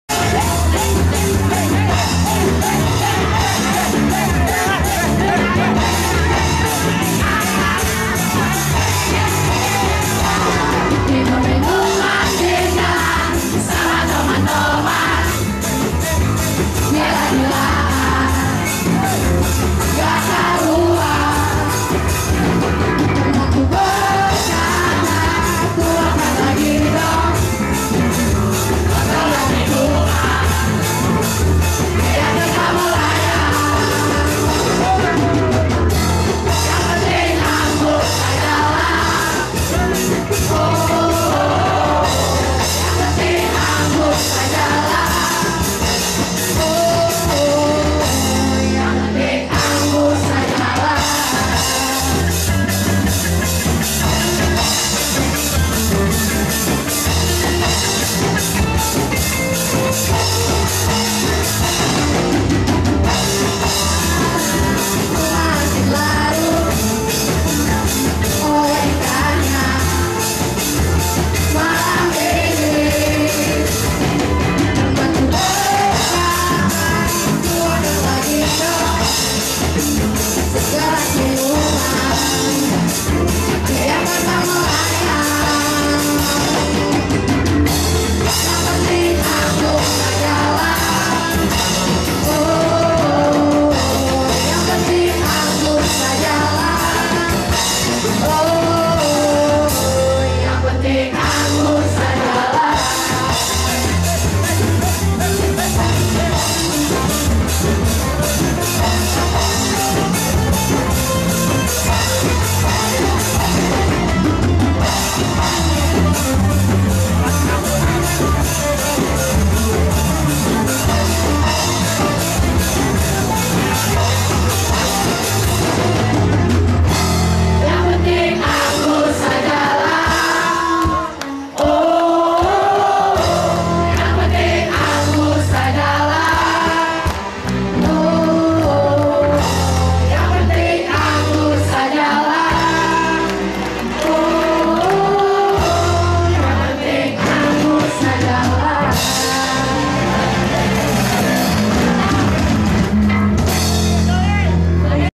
Dirumah aja bersama smpn 1 jalaksana selingan2 dalam festival pop song || NezalOne TV
dirumahaja menikmati tayangan dari pop song, untuk menghilangkan rasa jenuh dan sebagai penghargaan,apreasi dan motivasi atas kreatifitas untuk mengeksploasi kemampuan diri dalam acara festival pop song para siswa dan siswi kelas 9 SMPN 1 Jalaksana

Video-video tersebut sebagai pengobat rasa kagen dan rindu mereka pada sekolah dalam menghadapi copid19. Dimana mereka harus dirumahaja dalam melakukan segala aktivitas keseharian. Termasuk belajar juga dilakukan di rumah aja ( DIRUMAHAJA )